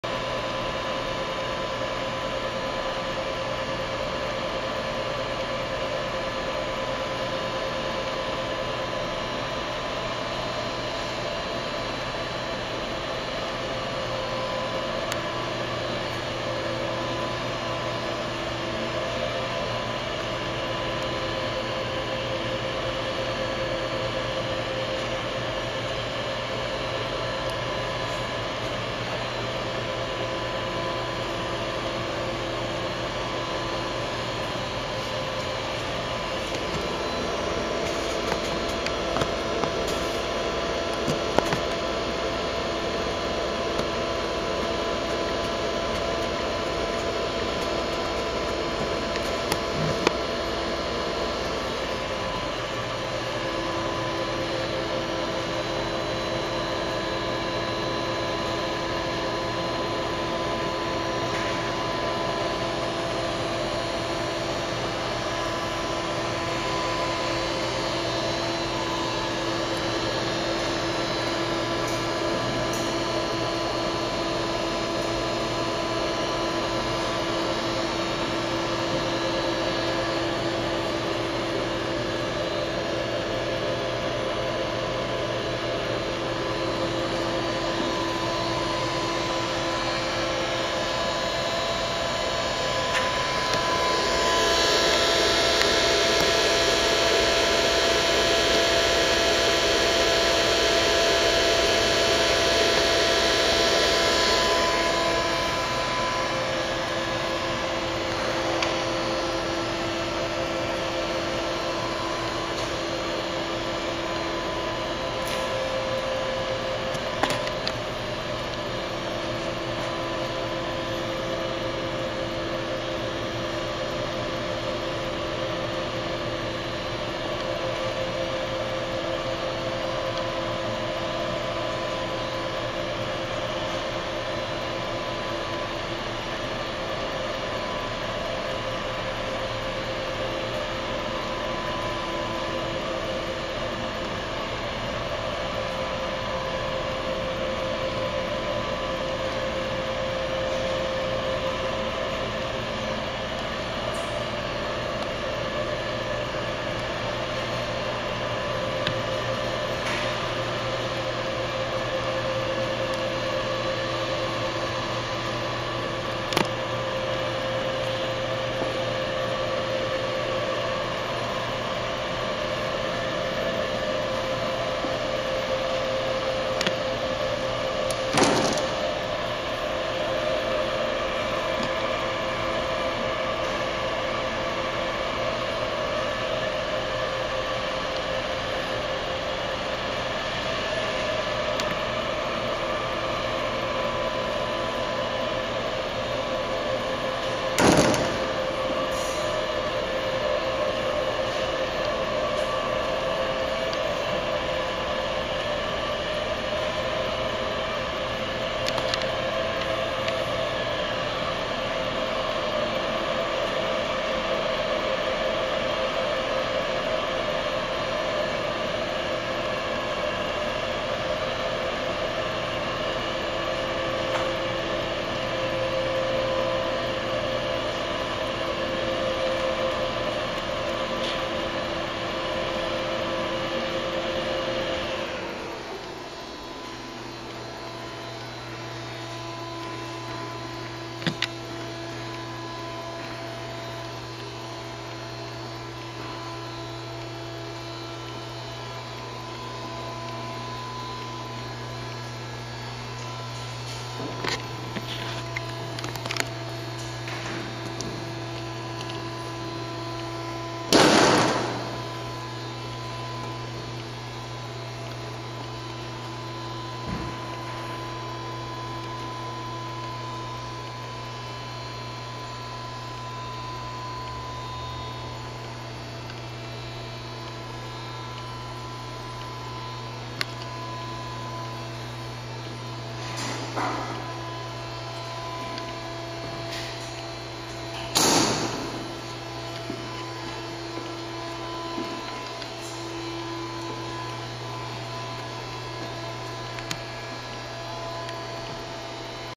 Adco Barrel Cam Cartoner Model 12BC90EC
This is a walk around video of a Adco Barrel Cam Cartoner Model 12BC90EC E8249 if you want more information about this please visit us to view our entire inventory please check us out